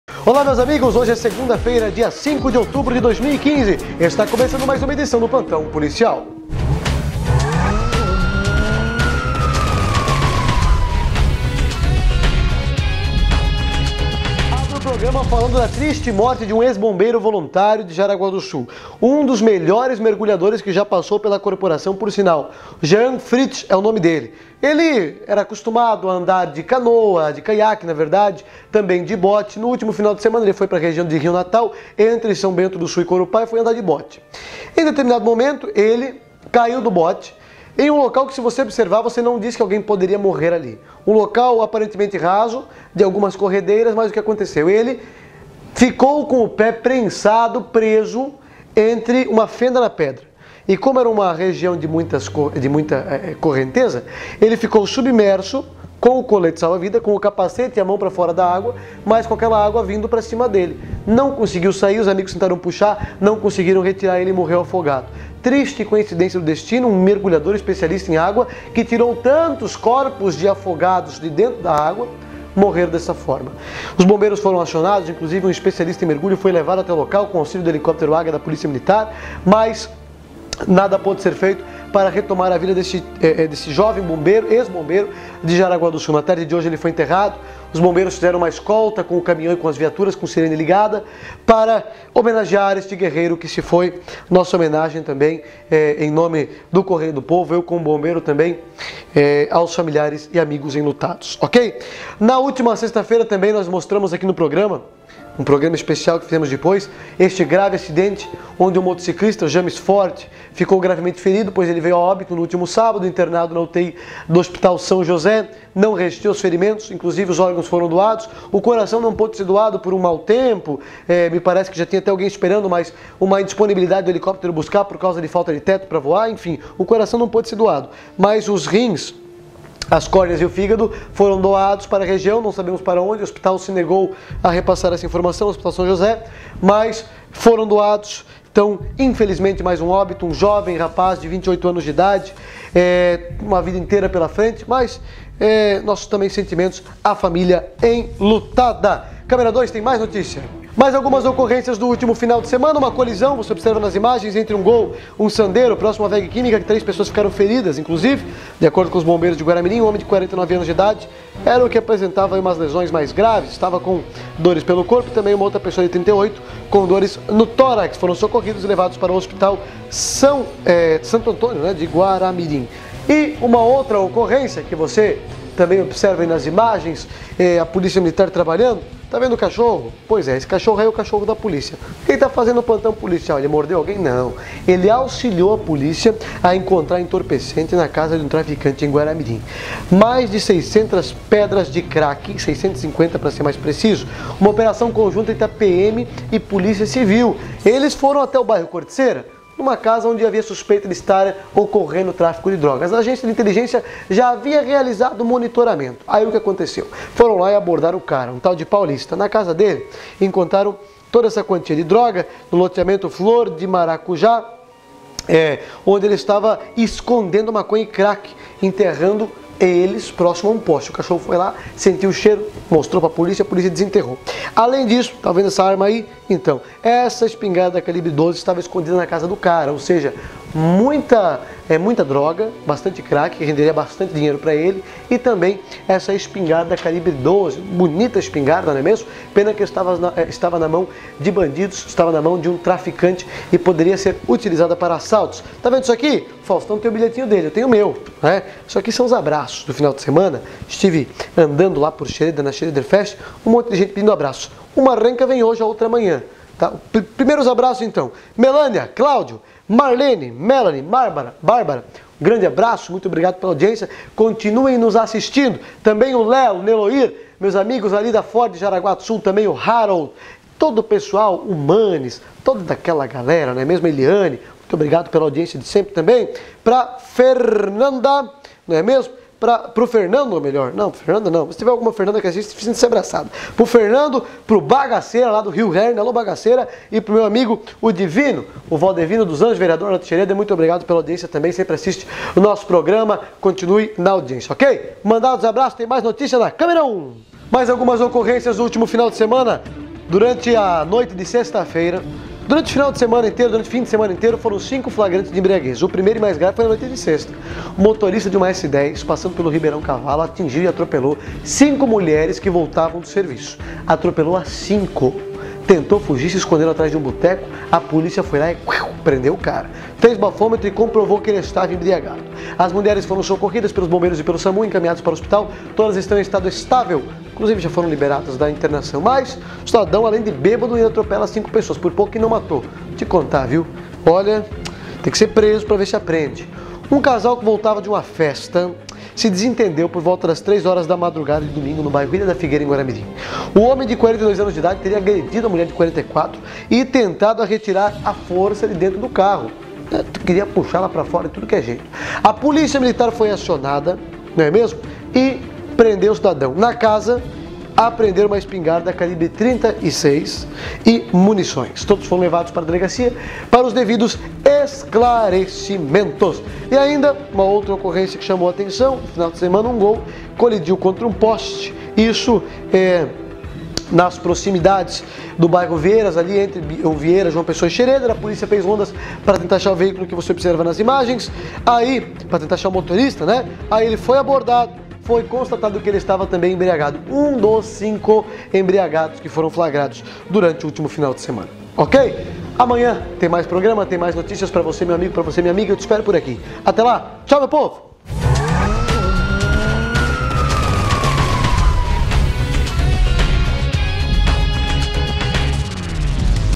Plantão OCP 05/10/15 (Duas mortes no fim de semana)
Vídeo - Hoje trazemos as principais notícias do final de semana com imagens e detalhes dos ocorridos. Entre os destaques, a morte de um ex-mergulhador dos bombeiros de Jaraguá do Sul que se afogou em São Bento do Sul, a morte do motociclista envolvido em acidente de trânsito em Guaramirim, atropelamento de cinco mulheres por um bêbado e traficante preso com arma em Guaramirim.

Assista!